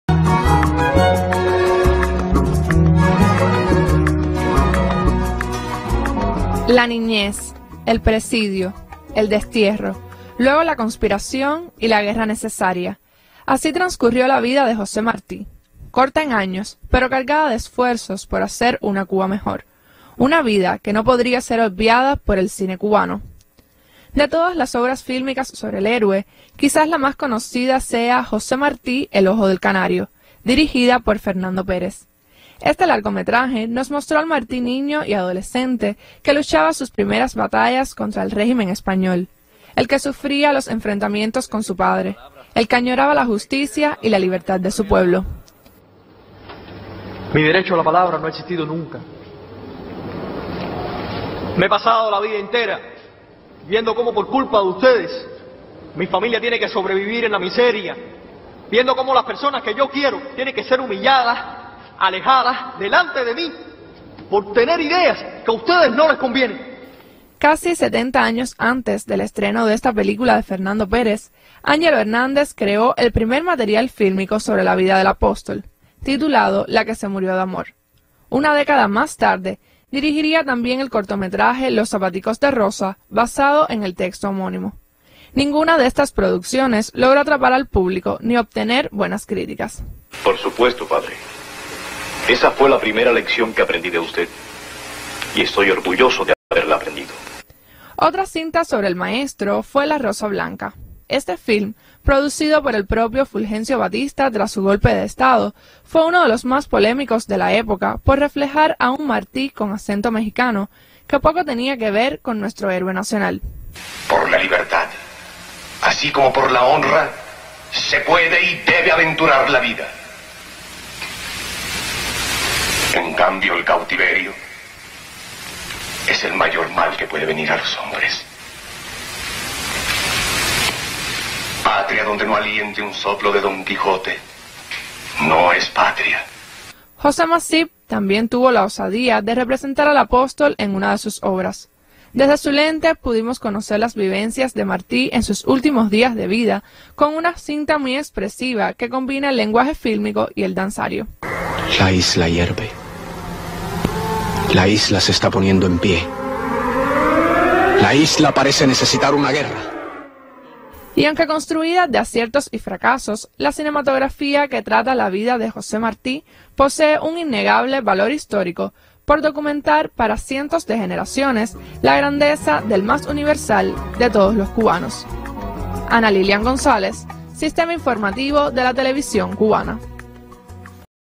La impronta de José Martí recreada por la cinematografía en Cuba
La aparición de JoséMartí en el cine en Cuba se divide fundamentalmente en 4 filmes y un cortometraje que narran los sucesos de su vida y obra. El valor de estas producciones destaca por su misión de acercar la historia del Apóstol al pueblo.